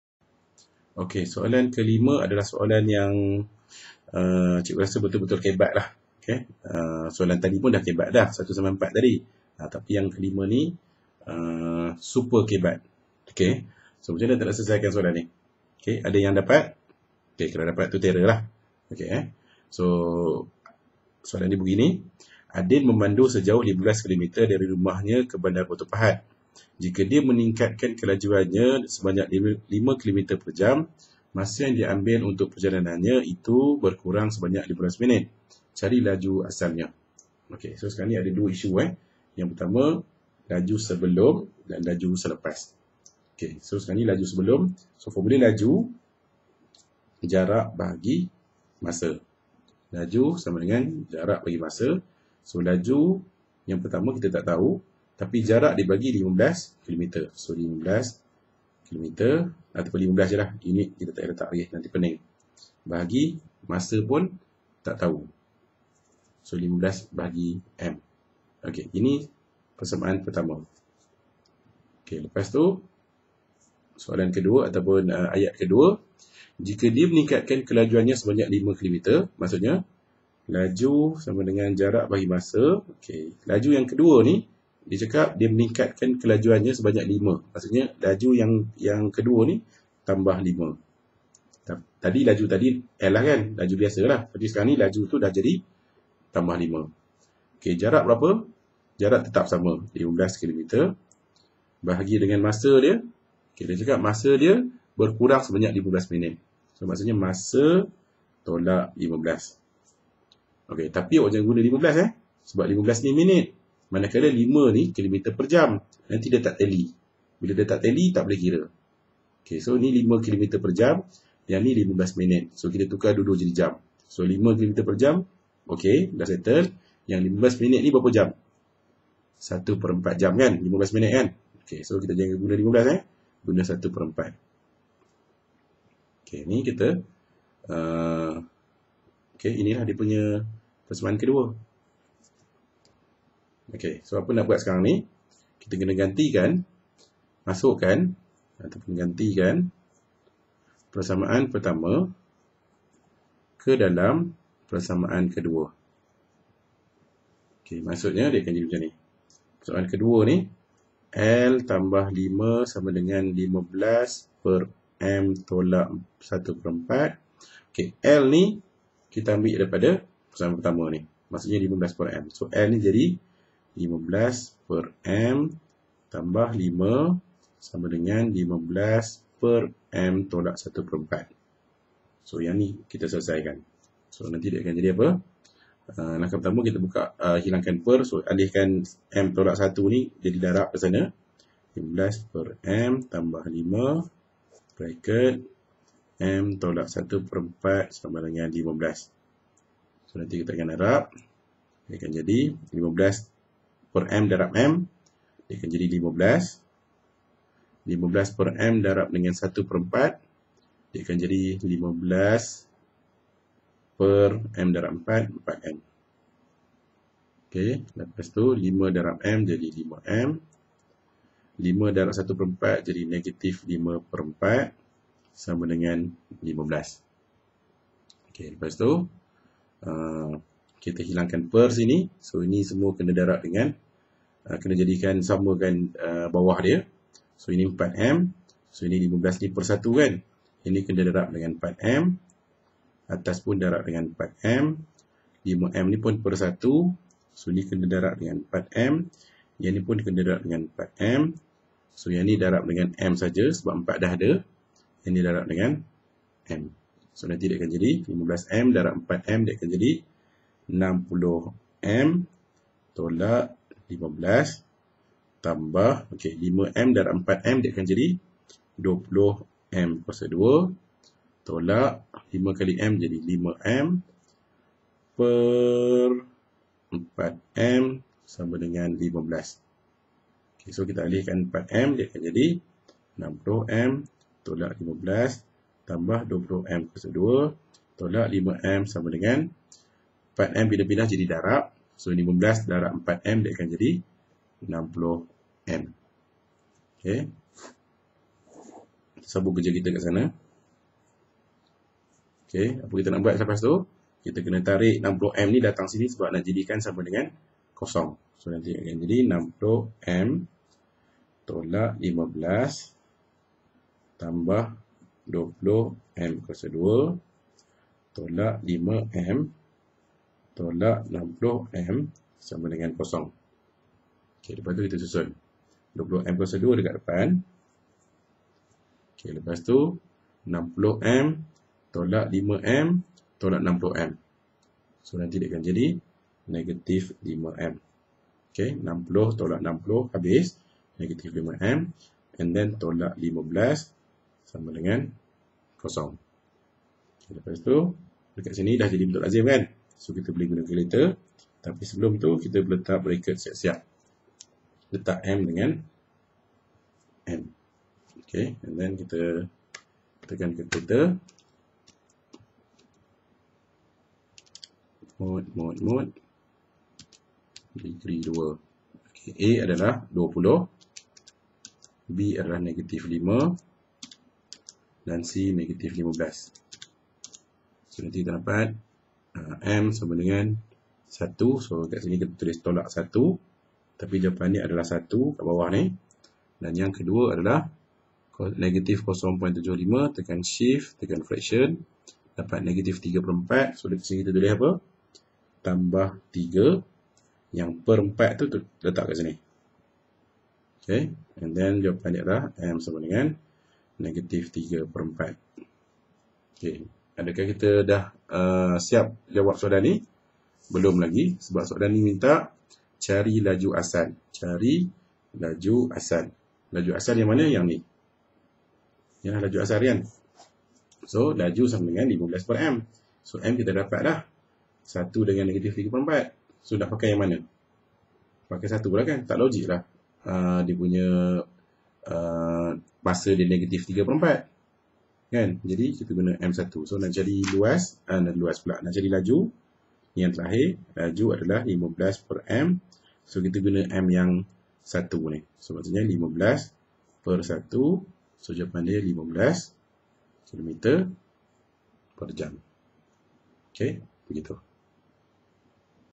F4. BAB 2: UNGKAPAN & PERSAMAAN KUADRATIK. BAH D: APLIKASI PERSAMAAN KUADRATIK. NO: 5
VIDEO UNTUK BUKU MATHS F4 MODUL 1 PAGE 18 BAGI CHANNEL TELEGRAM MATHS F4 CIKGU AZIZUL. HUBUNGI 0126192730 JIKA INGIN TURUT SERTA . FREE OF CHARGE